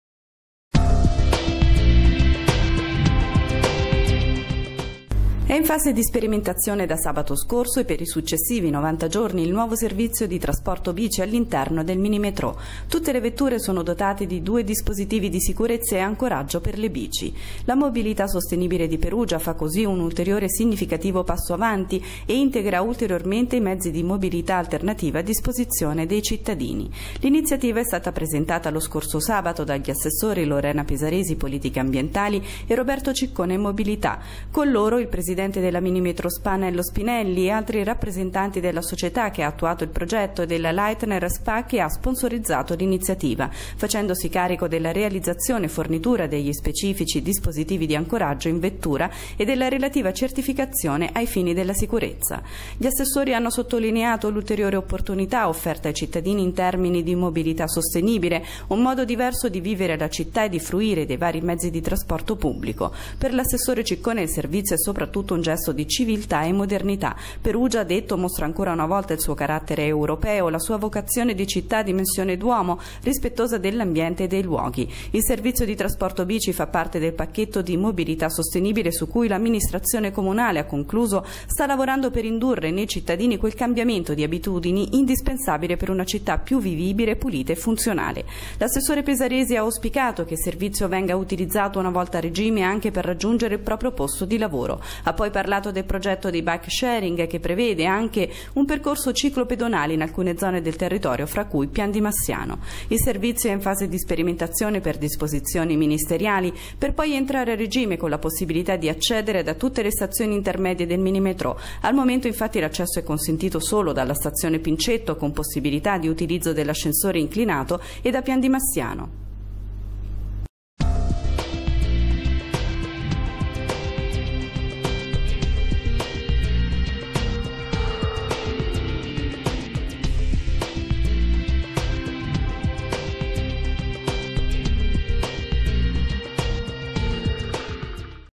BICI IN MINIMETRO'
IL NUOVO SERVIZIO E' STATO INAUGURATO DAGLI ASSESSORI PESARESI E CICCONE: "LA GIORNATA DI OGGI E' UN SEGNO DI GRANDE CIVILTA' E INNOVAZIONE"